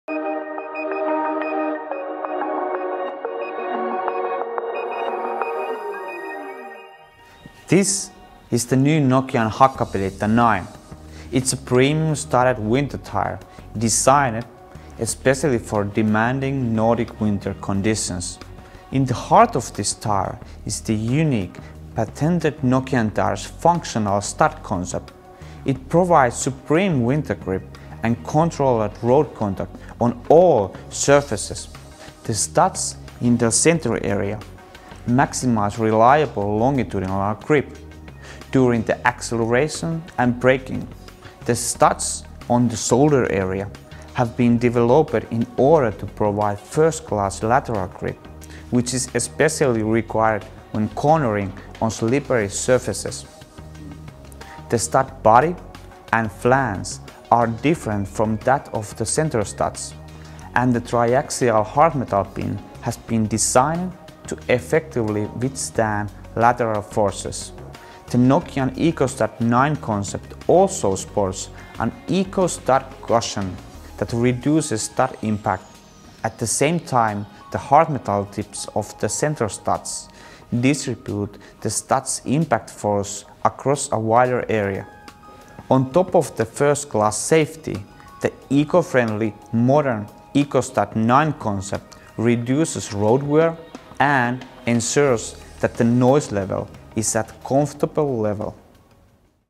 Tyre technology in 60 seconds: Nokian Hakkapeliitta 9's functional studding
Nokian Hakkapeliitta 9 and its patented double stud technology provides excellent braking and acceleration grip and also superior grip in curves and when passing on both icy and snowy roads. But how does the functional studding work?

You can find all our winter tyres for passenger cars